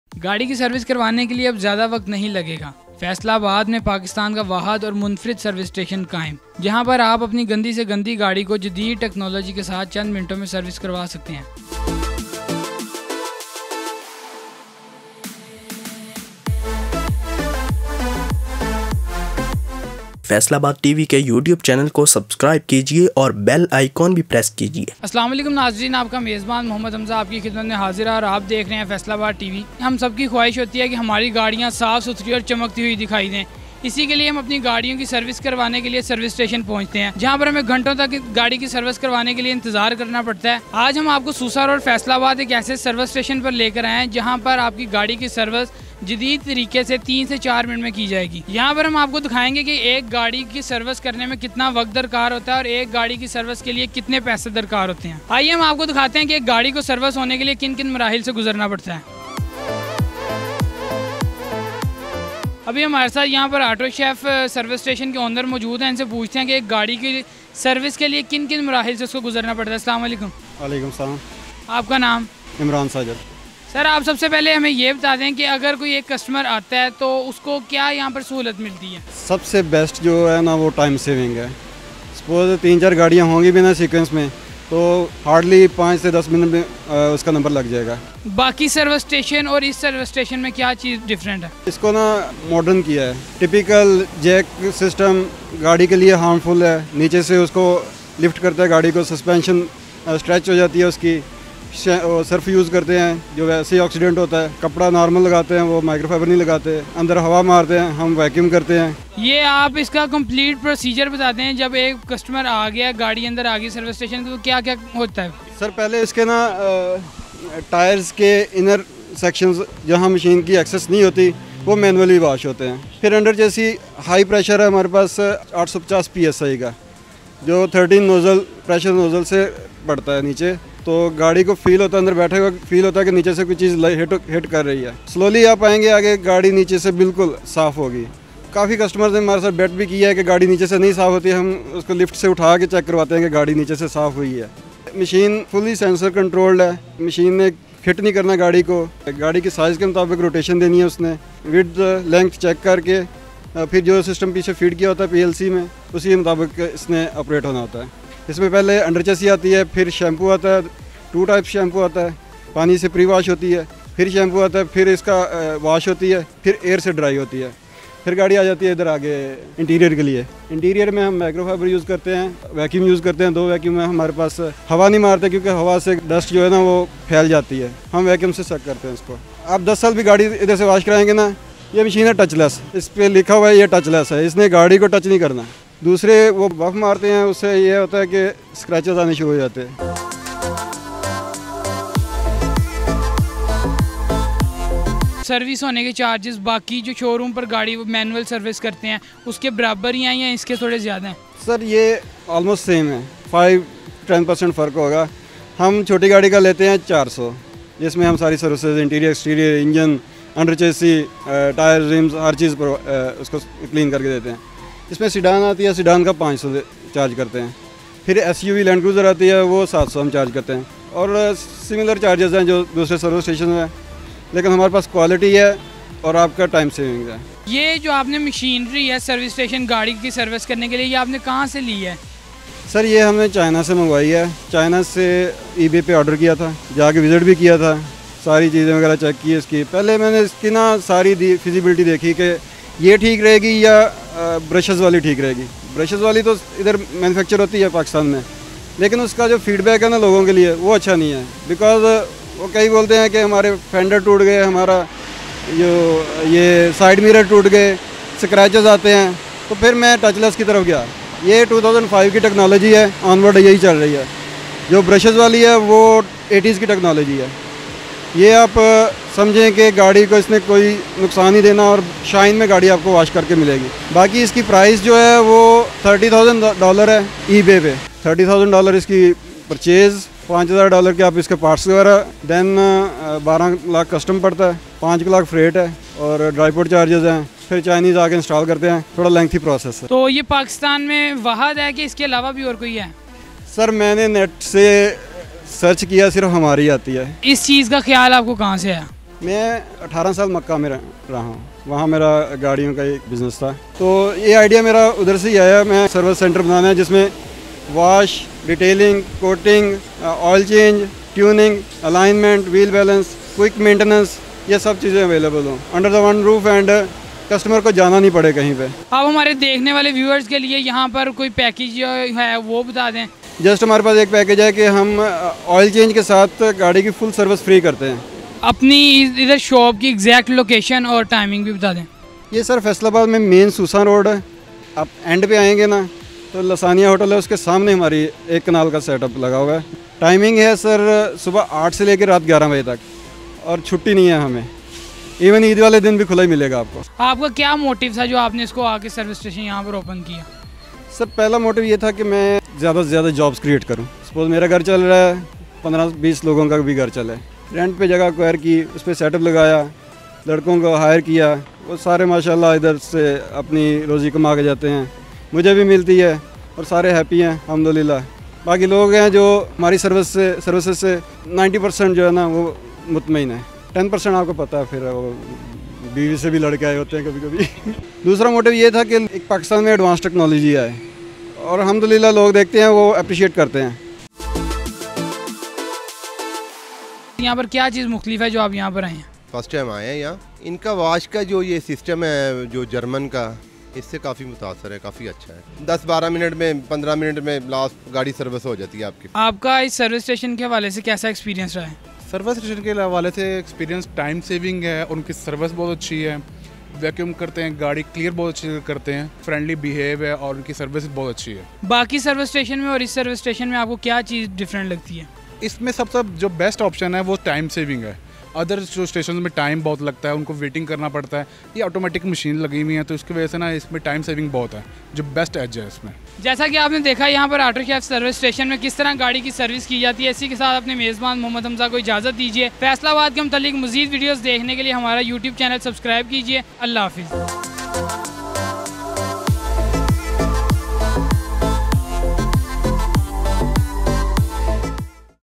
Automatic Car Wash In Pakistan That Washes Your Car In less Than 4 Minutes | F TV
AUTO CHEF is the latest Car Wash service station in Susan Road Faisalabad which has an Automatic car wash system. It is equipped with the latest machinery that washes the car in less than 4 minutes and it costs 300 rupees. Watch the video for more details.
WARNING⚠️
Reposting any part of this Video will be considered an infringement of Copyright©️